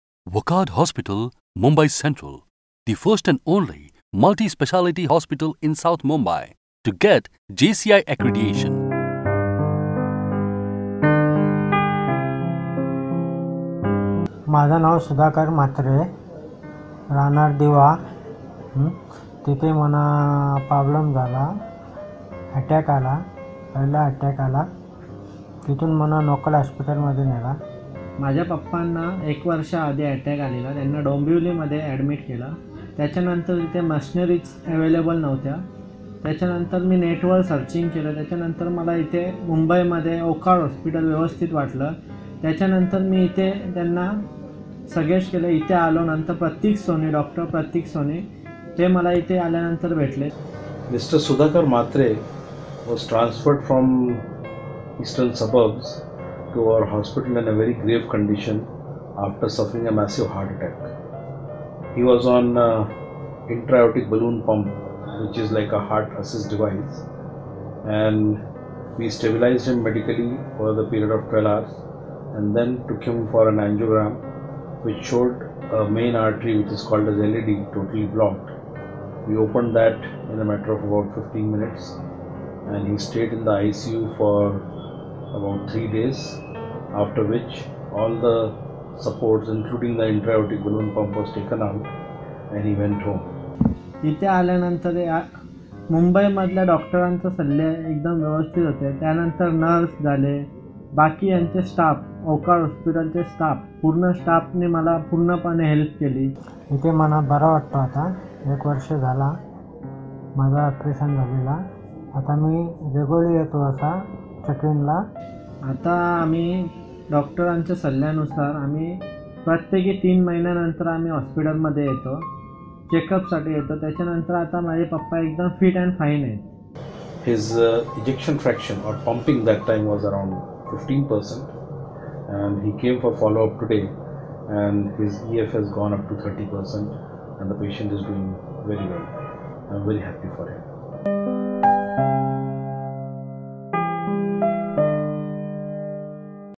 Sudhakar Mhatre Angioplasty Treatment by Dr Pratik Soni | Wockhardt Hospitals, Mumbai Central
Sudharkar Mhatre approached Wockhardt Hospitals, Mumbai Central with hopes after suffering a massive heart attack. He was stabilised with an Intra-aortic balloon pump. Eventually, an Angioplasty procedure was done and now his ejection fraction has gone up to 30%.